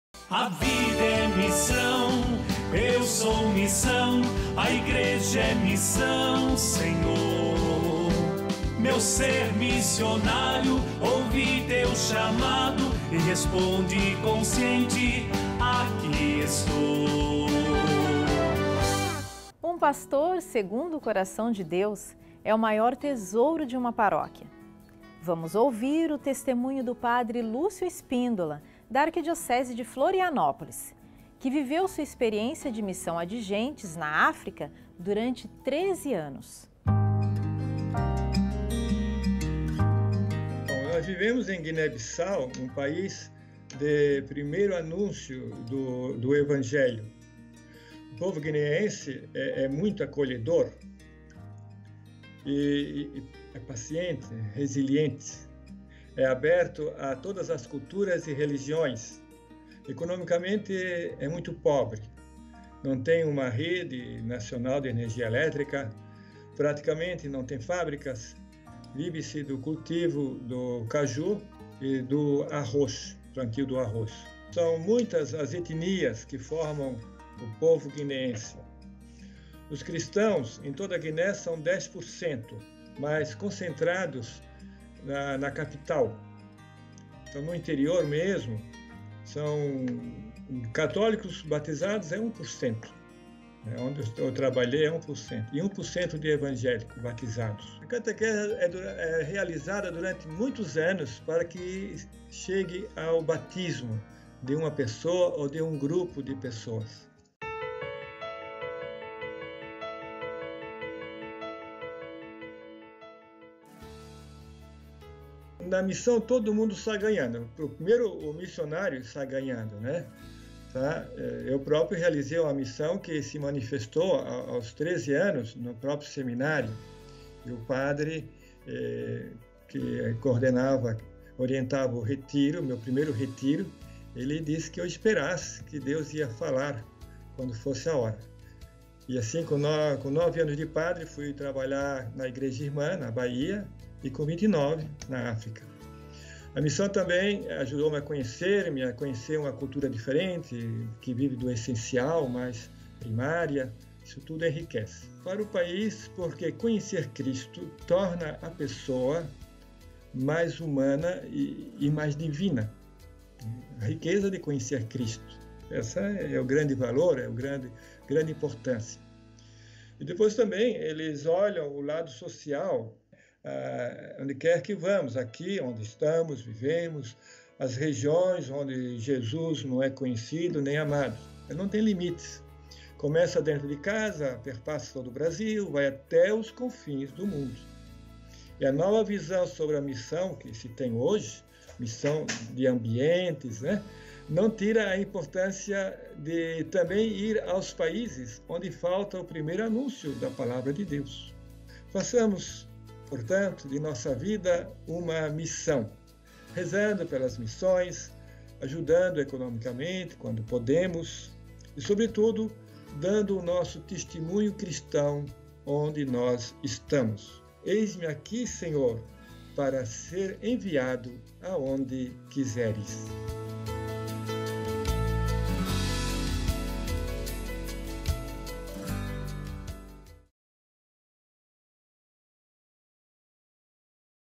6º Dia - A VIDA MISSIONÁRIA DOS MINISTROS ORDENADOS
Ser diácono, padre ou bispo missionário, significa colocar a sua vida a serviço. Estar disponível para viver em lugares onde haja necessidade da luz do Evangelho. Eis-me aqui, envia-me. Apresentamos o testemunho do padre Lúcio Espíndola, da Arquidiocese de Florianópolis (SC), que viveu sua experiência de missão ad gentes na África durante 13 anos.